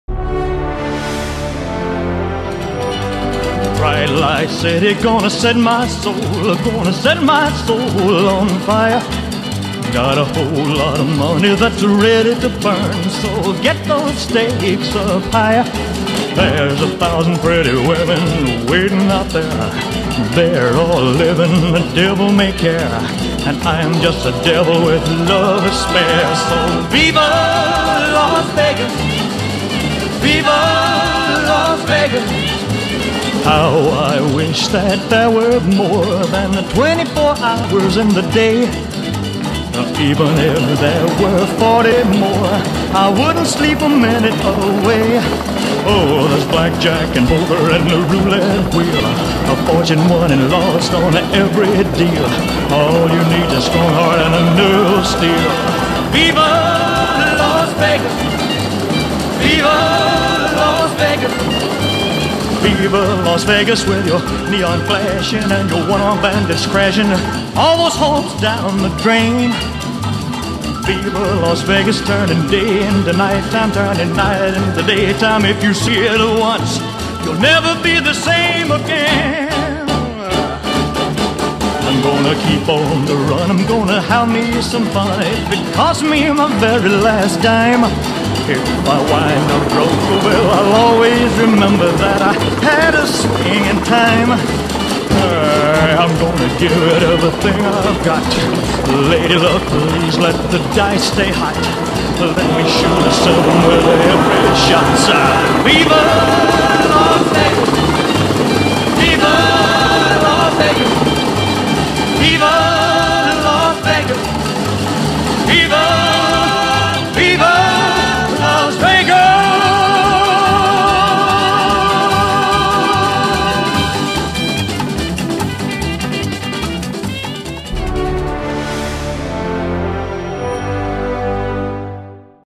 2003-10 USA 2003 Las Vegas helicopter at night Elvis
Spectacular views of Las Vegas at night from a helicopter with Elvis singing the timeless classic! Anita and Mark end their USA 2003 holiday in style. FULL MOVIE